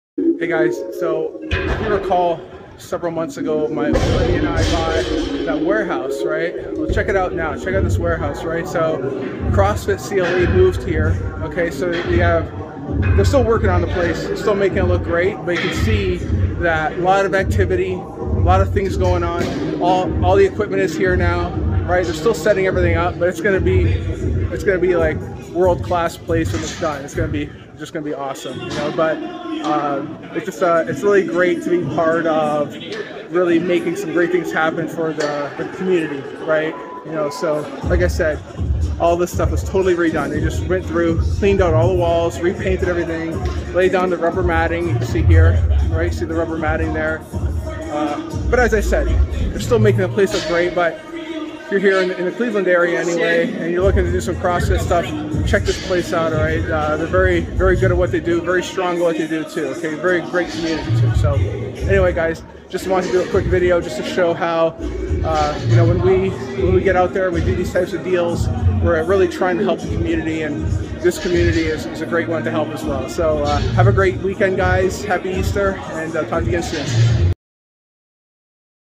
Warehouse Update
Recently, my buddy and I bought this amazing warehouse. The guys at CrossFit CLE moved their community into the space and transformed it into a badass fitness hub 💪🏋️‍♀️

🏋️‍♂️ When we invest in real estate, we also invest in the community. Join us in building better spaces together!